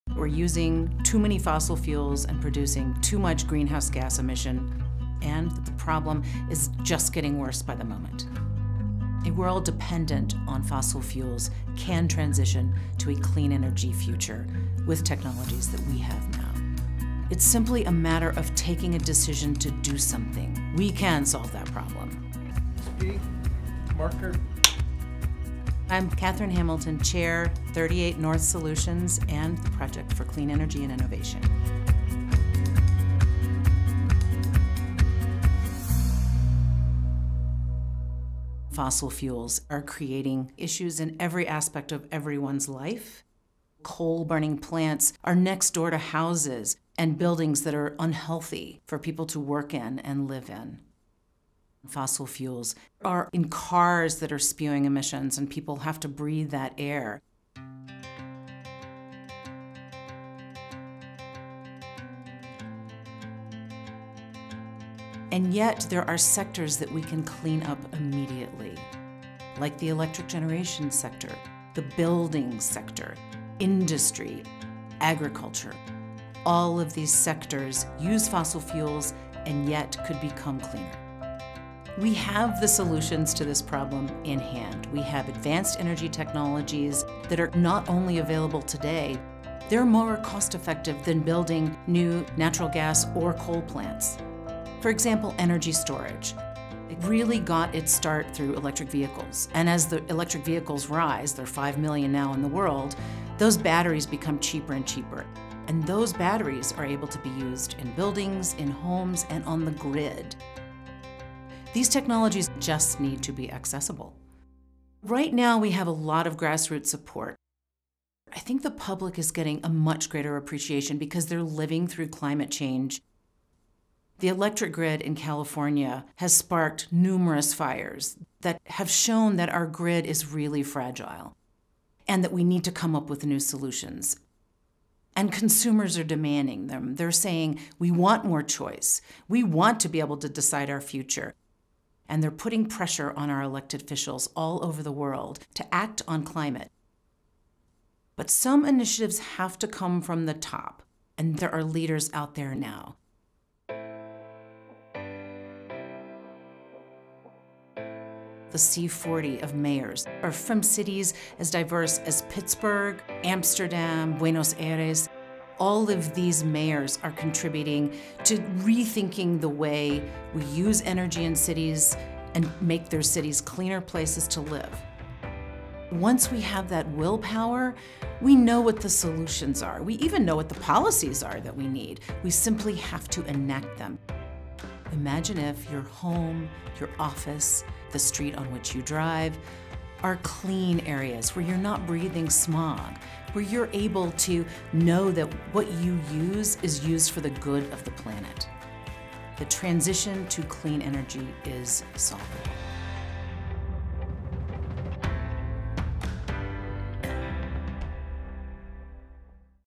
Dependence on fossil fuels is Solvable | Katherine Hamilton
Dependence on fossil fuels is Solvable. Katherine Hamilton explains how we can transition to a clean energy economy through the advancement of technology and leadership to end dependence on fossil fuels like coal, oil, and gas.

Katherine Hamilton is the Chair of 38 North Solutions, a boutique consulting firm that provides a suite of business strategy, public policy, and communications services to innovative businesses and organizations. She currently serves as Co-Chair of the World Economic Forum’s Future of Energy Global Future Council and was formerly the President of GRID Alternatives Mid-Atlantic.